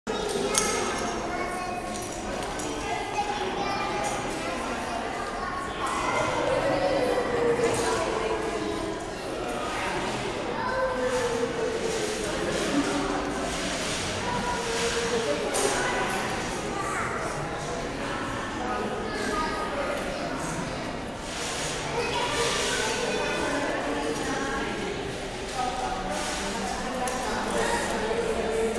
Fallingwater in Lego
Fallingwater done in Legos at the Building Museum in Washington, D.C.